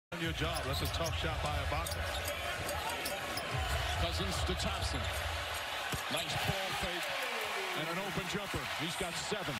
NBA, playoff 2019, Raptors vs. Warriors, Round 4, Game 4, Move 18, Klay Thompson, 2 pointer
Toronto Raptors - Golden State Warriors, Game 4, 07/06/2019, quarter 2
Klay Thompson, 2 pointer